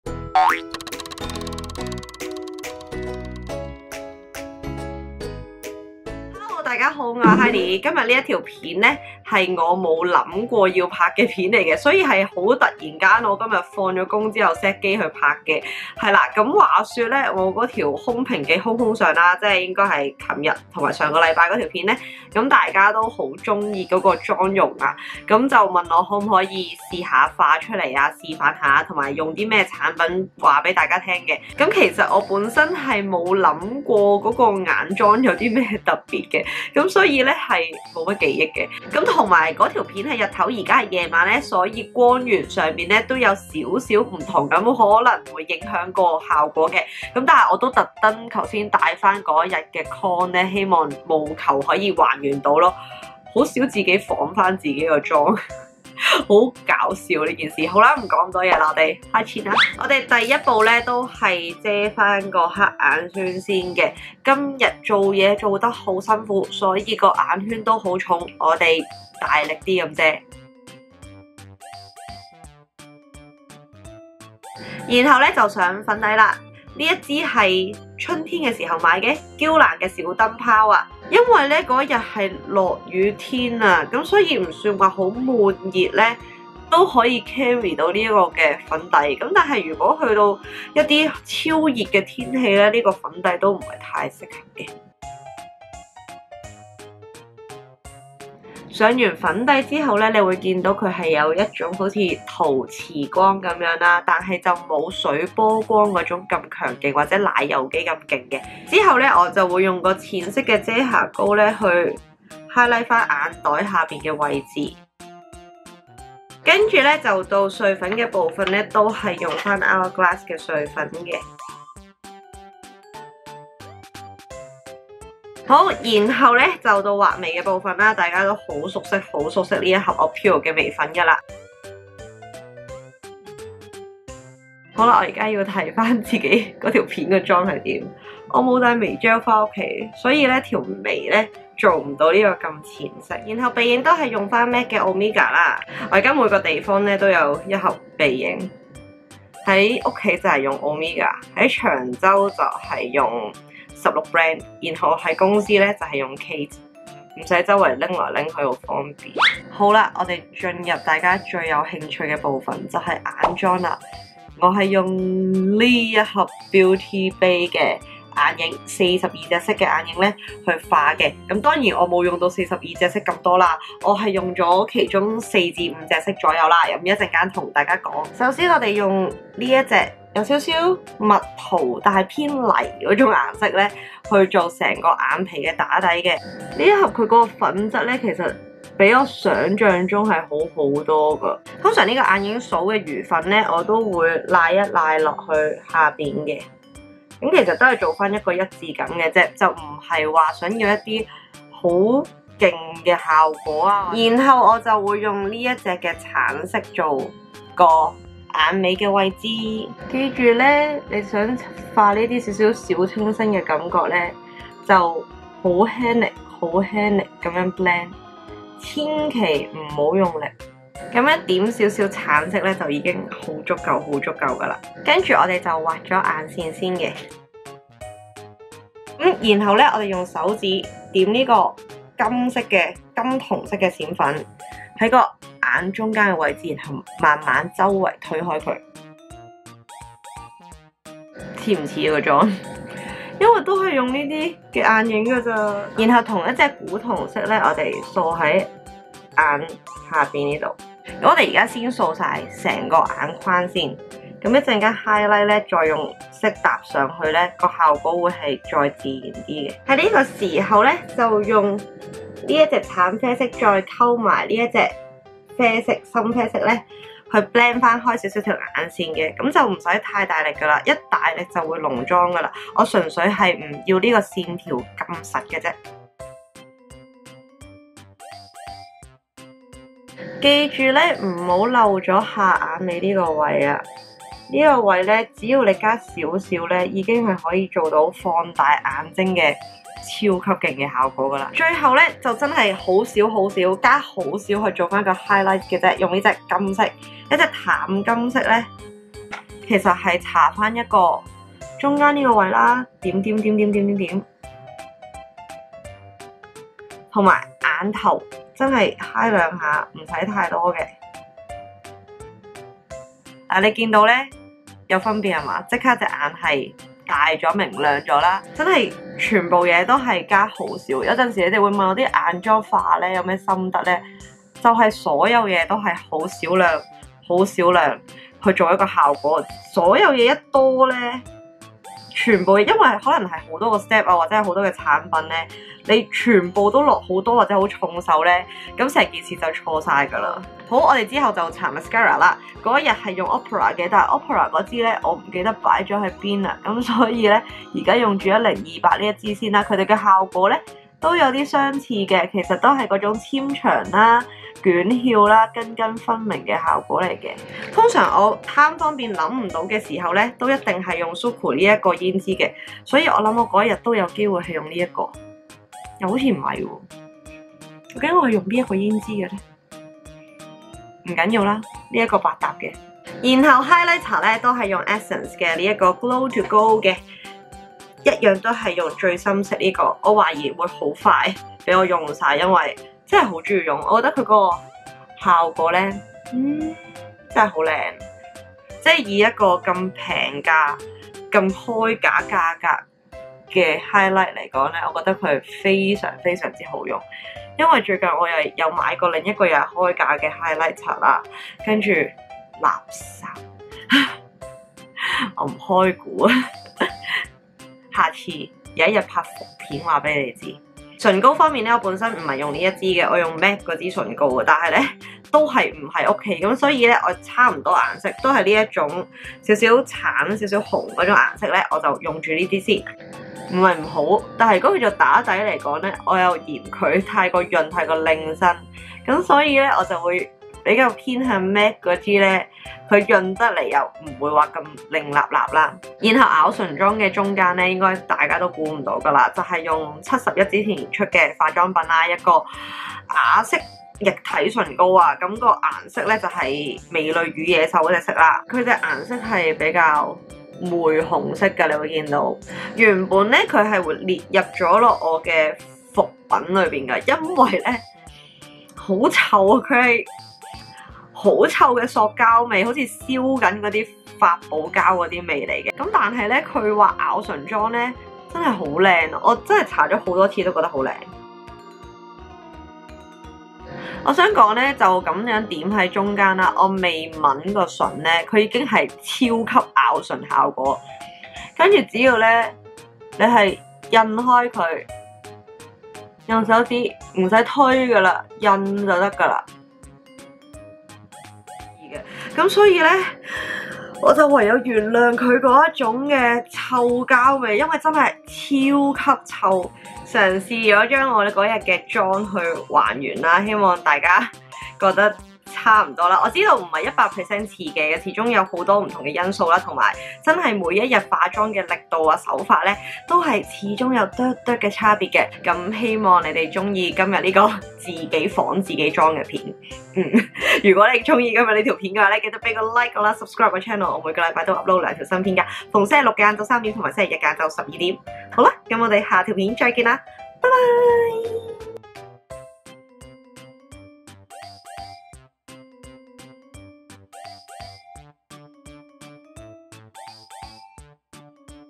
♥ 化妝 ♥ 仿自己妝？嘗試化空瓶記妝容 || Dottie Hidee
♥ 聊天時間 ♥

我盡量了XDDDDD
大致上係咁化啦~
其實我好多時都係無咩藍圖
最多諗下想要咩色嘅眼妝
就用選妃方式去揀用邊盒眼影XD

❋ 膚質 - 沙漠暗沉敏感肌
❋ 身高 - 153cm
❋ 體重 - 44kg

♥ 今集內容提及:

❋ Maybelline - Instant Age Rewind
❋ Guerlain - L'Essentiel Natural Glow Foundation
❋ NARS - Radiant Creamy Concealer
❋ A'pieu - Shaping Brow Kit Dark Brown
❋ Hourglass - Veil Translucent Setting Powder
❋ MAC - Single Eyeshadow
❋ Beauty Bay - Origin 42 Colour Eyeshadow Palette
❋ Moteliner - Eyeliner Dark Brown
❋ 1028 - 飛激長瞬翹防水睫毛膏
❋ Suqqu - Blush #105
❋ Essence - Glow to Go Highlighter Palette
❋ Maybelline - 琉璃誘光精油唇膏 #15
❋ 7-11 - Liquid Lipstick Beauty and the Beast (?)